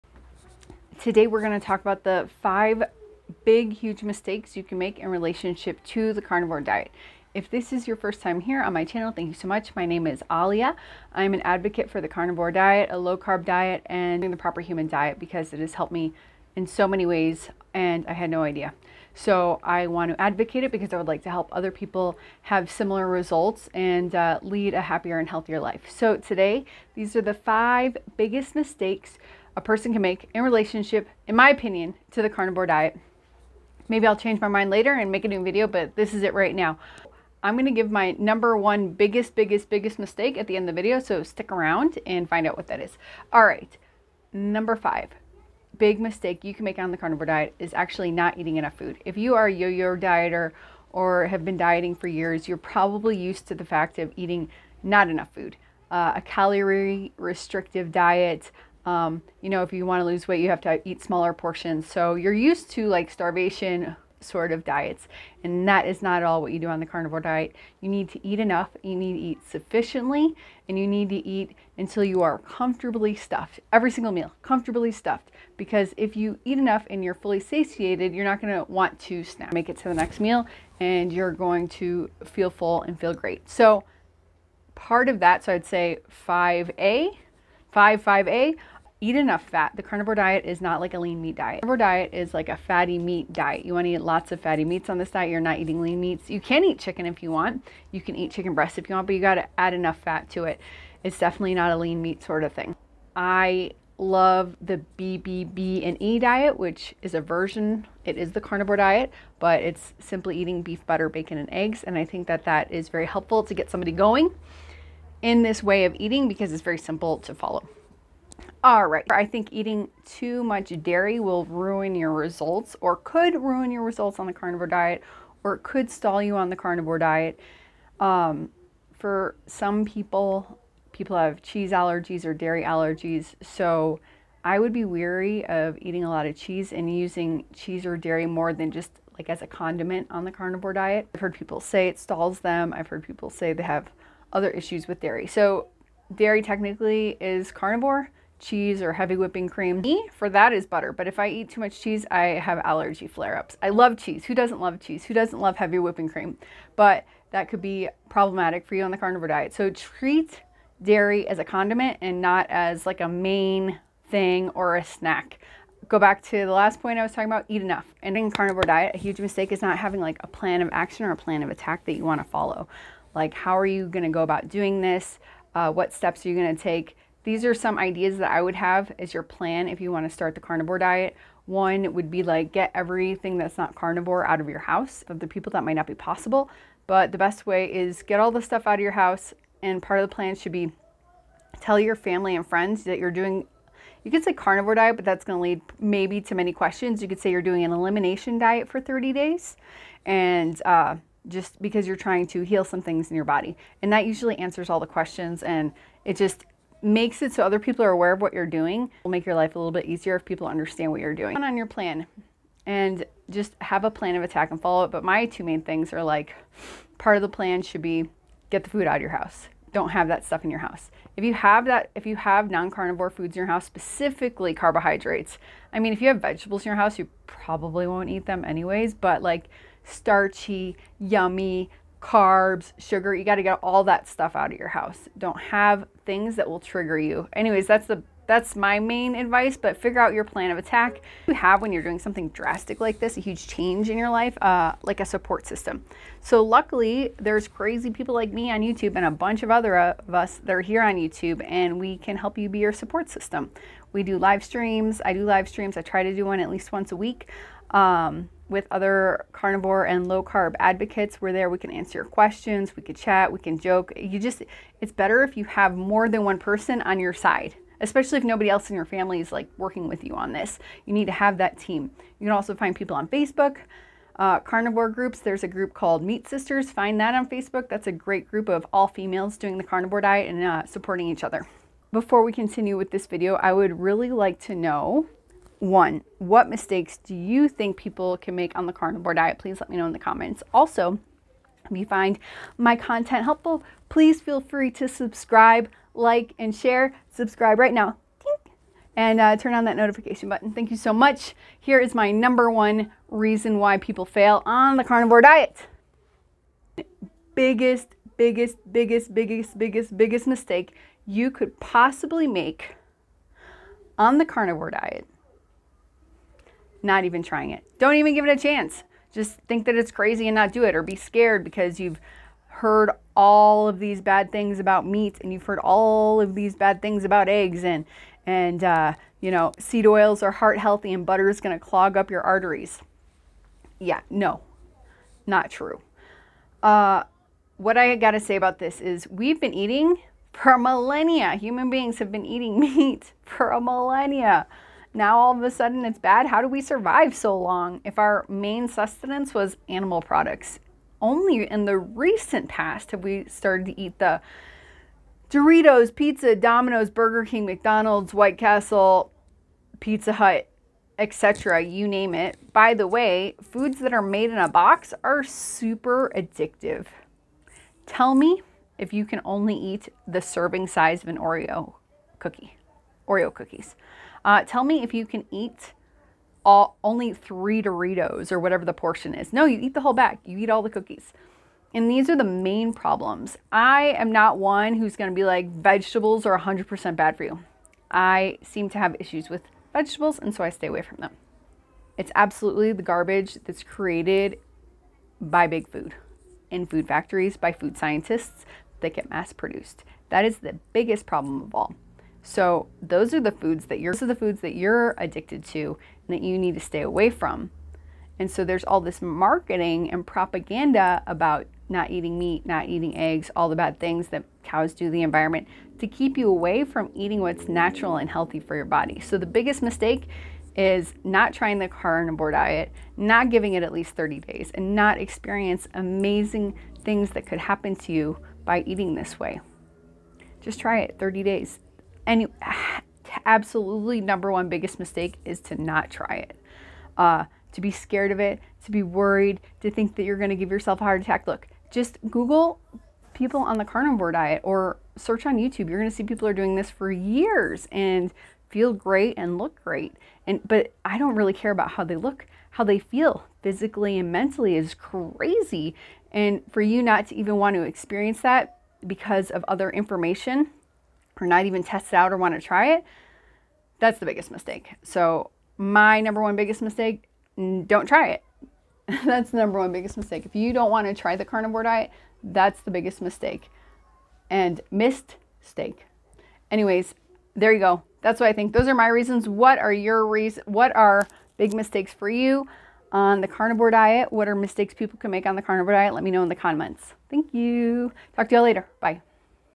5 PITFALLS of the CARNIVORE DIET: The Critical Mistakes to Avoid for Success
Don't make these carnivore diet mistakes.

5.  Not eating enough / not eating enough fat
4. Too much cheese and dairy
3. No plan of attack for success
2. No support system.
1. The biggest mistake that you could make on the carnivore diet.